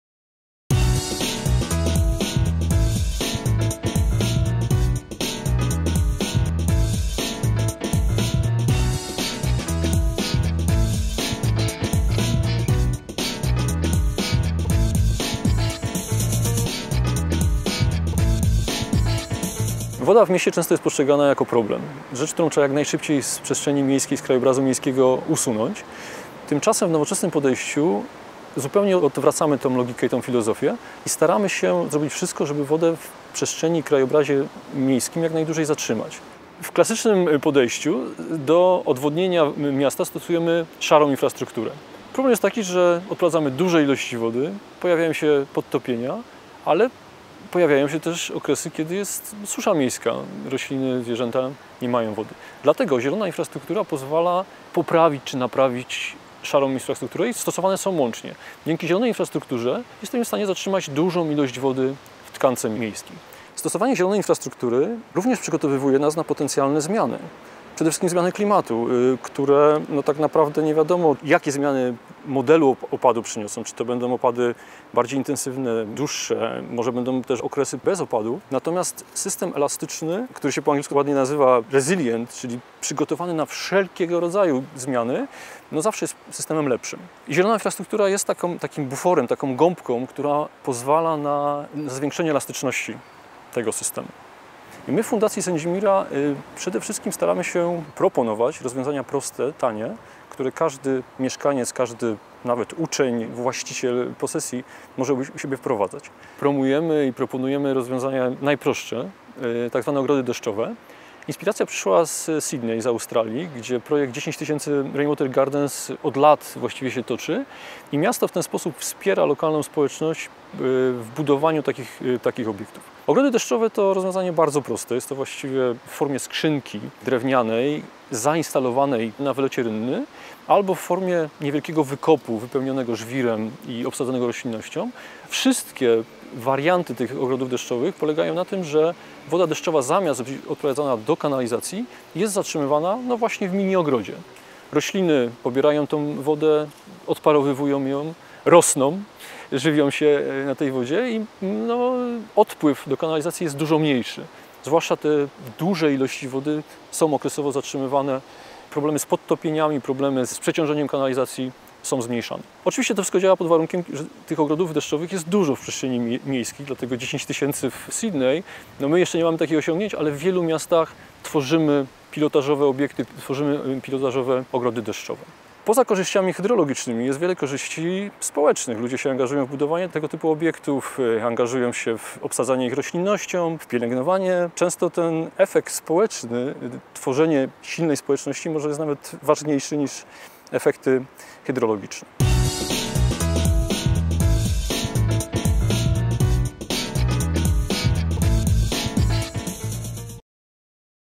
Zmiany klimatu w naszych miastach - woda w mieście
Dr Tomasz Bergier z Fundacji Sendzimira, wyjaśnia jak ważne jest zatrzymanie jak największej ilości wody w przestrzeni i w krajobrazie miejskim. Doskonale nadaje się do tego zielona infrastruktura.